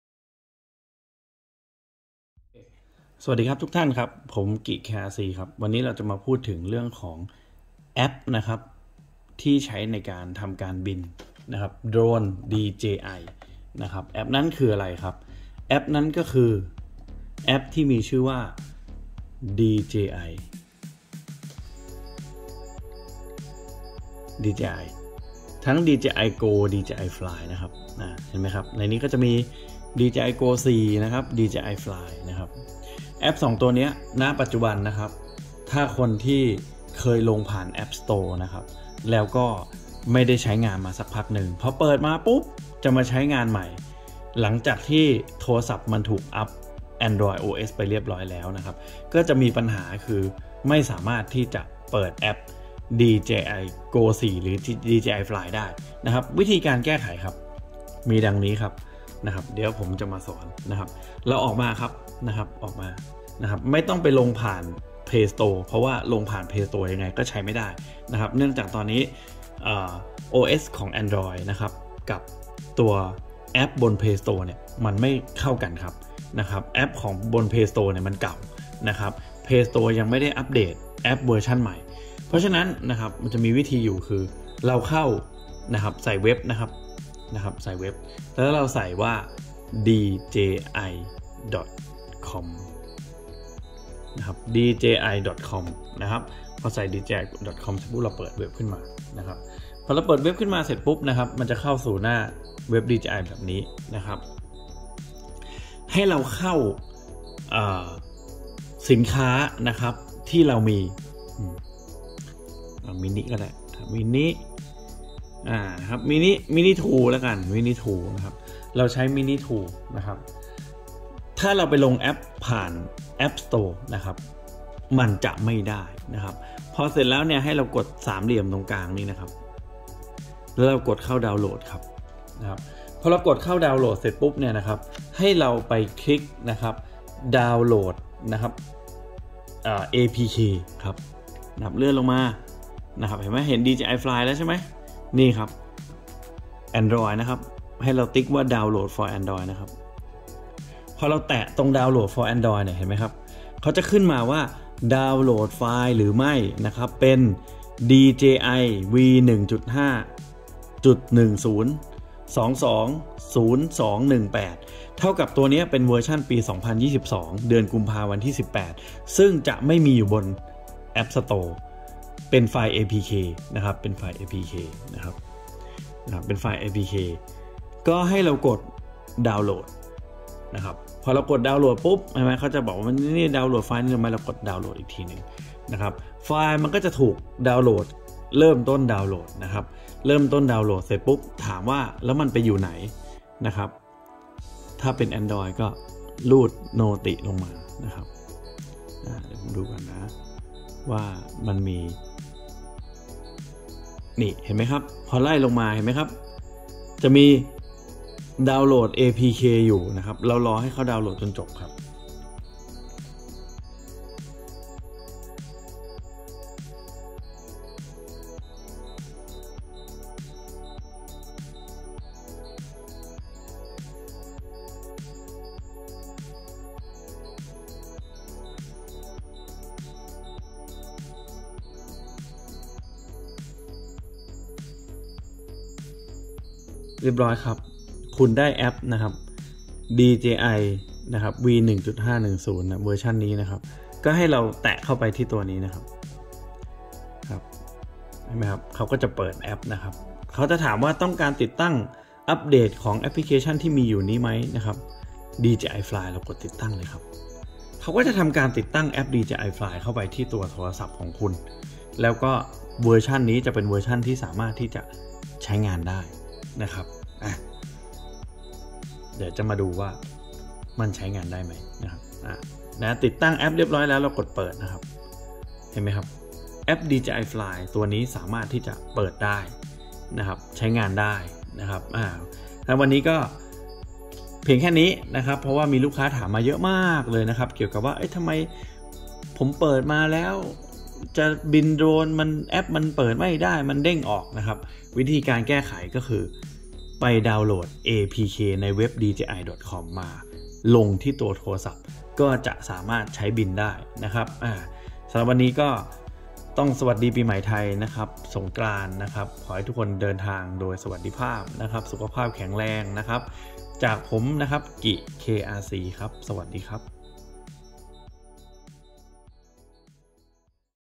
App DJI เด้ง !!!
วันนี้ KRCSHOP มาแนะนำวิธีลง APP DJI FLY และ DJI GO สำหรับโทรศัพท์และ Tablet Android OS ครับ

KRCSHOP | DJI Dealer & GADGETS Store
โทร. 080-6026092

👉🏻 ผู้เชี่ยวชาญ โดรน และ แกดเจ็ต 10 ปี ! มีบริการหลังการขาย ศูนย์ซ่อมอะไหล่แท้